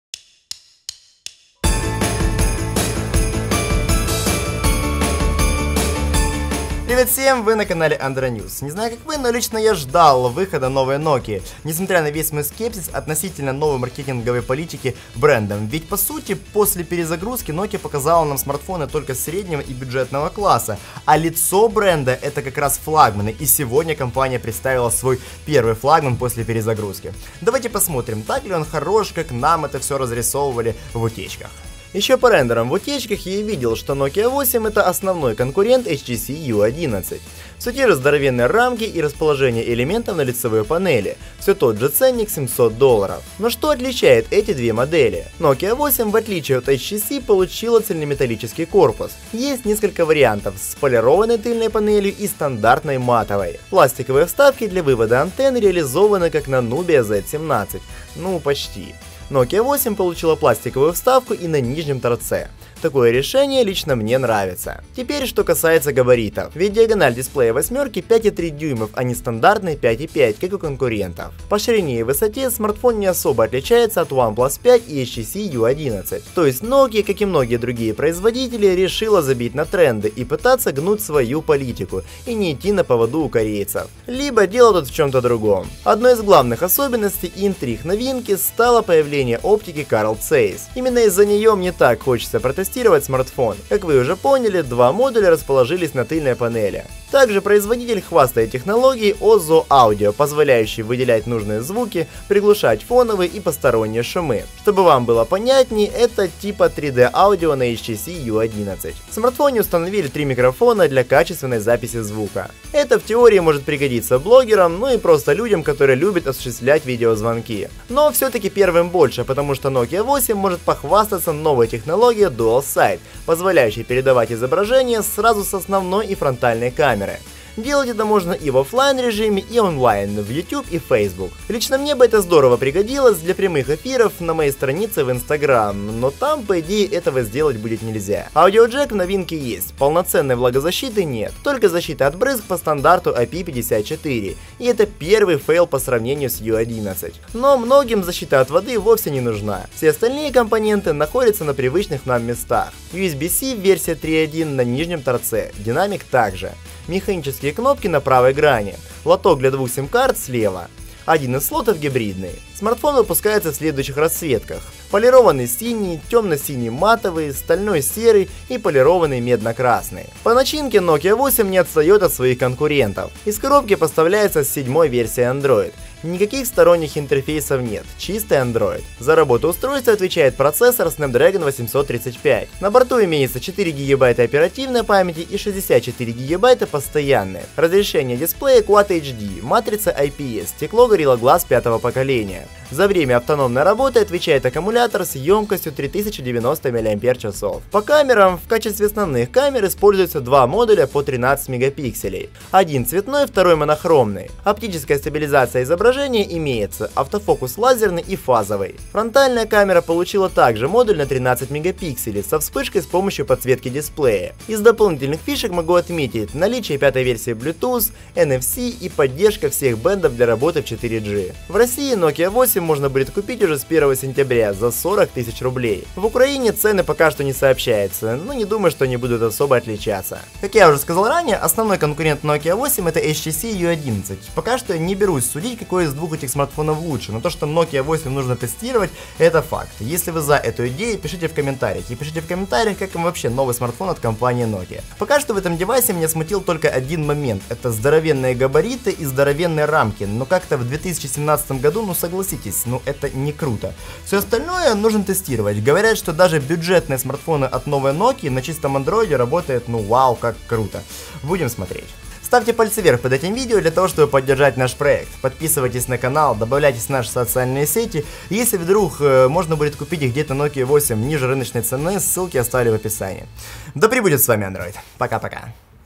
Nokia 8: хочется и не хочется...
Nokia 8 представлен. Отзывы, обсуждение, review, мнение.
⇒!Рекомендую! Сделай свои покупки на Aliexpress Безопасными, установи помощника

Не знаю как вы, но лично я ждал выхода смартфона Nokia 8. Несмотря на весь мой скепсис относительно ведения новой маркетинговой политики брендом.
Ведь по сути, после перезагрузки, Nokia показала только смартфоны начального и среднего класса. Оценивают лицо компании по флагманам, потому что это максимум на что способен бренд.  И сегодня я выскажу свои первые мысли о первом флагмане новой Nokia.

⇒ КУПИТЬ Nokia 8 можно тут:
Ожидается...

С ув. Андрей Ковтун!